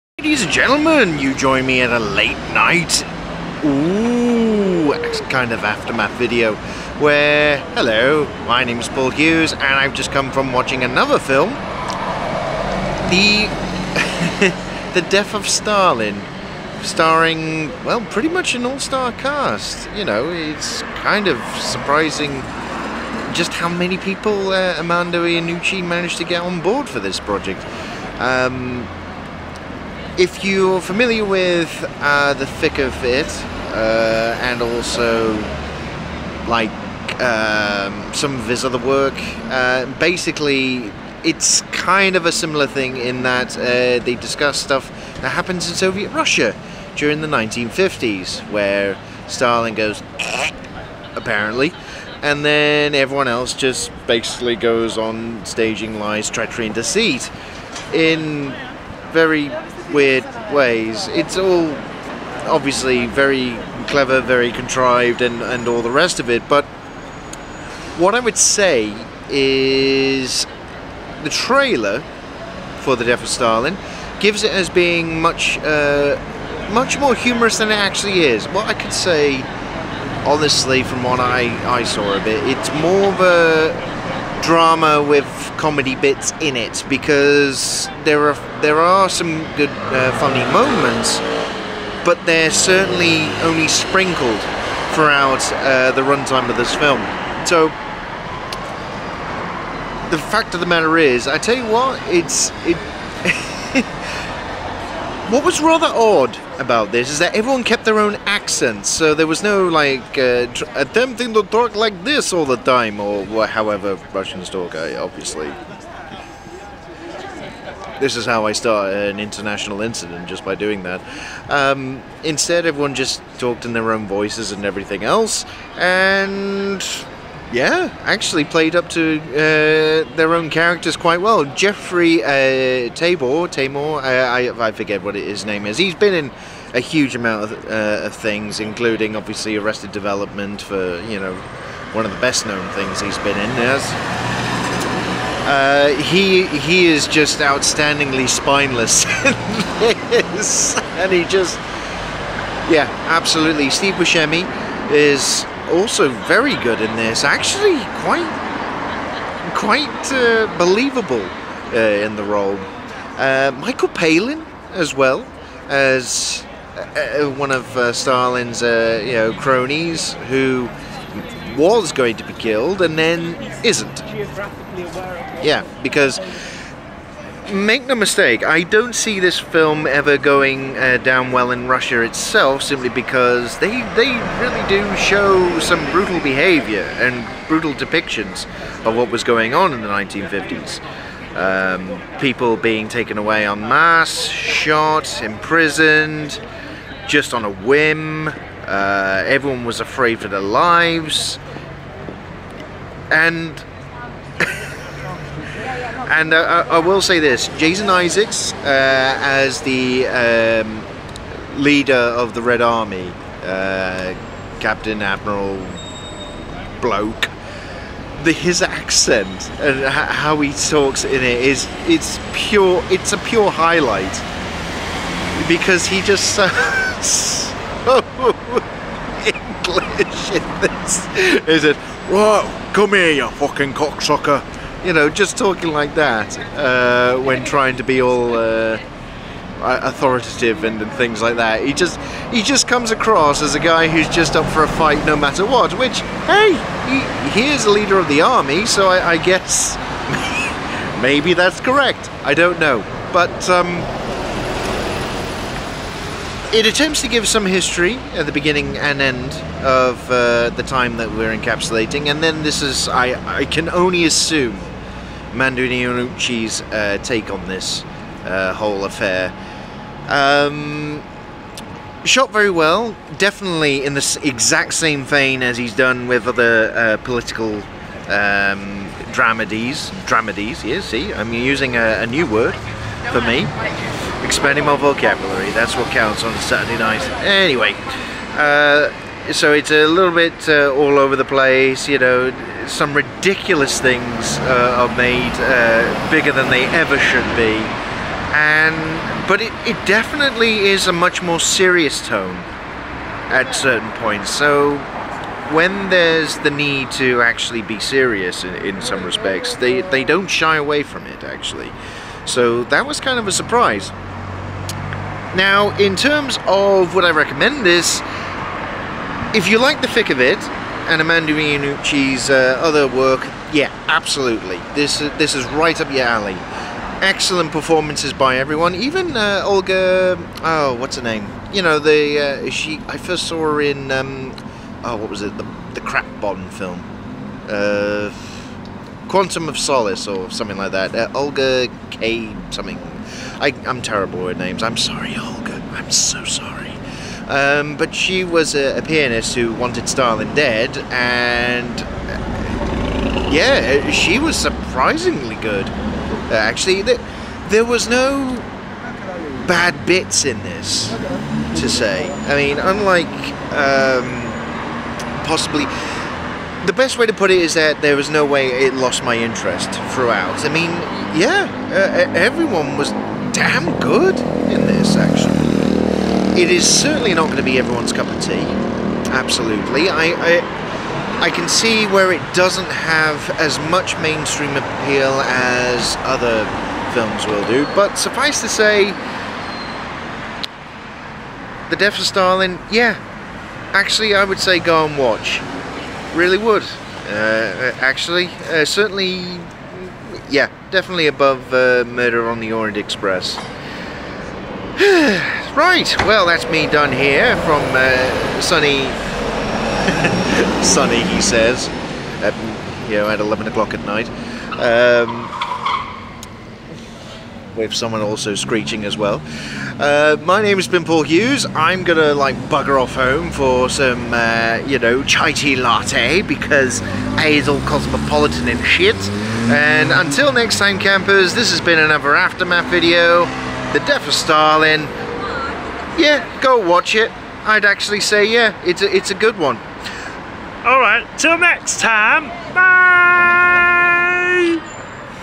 The Death of Stalin Aftermath
Not content with seeing one new release for moaning about, I went along again to the cinema to witness the Death of Stalin, the new political drama with comedy bits in like his other stuff. With an all star cast, will it make us rise up in Triumph or revolt against the oppressive meh on display? Let's find out!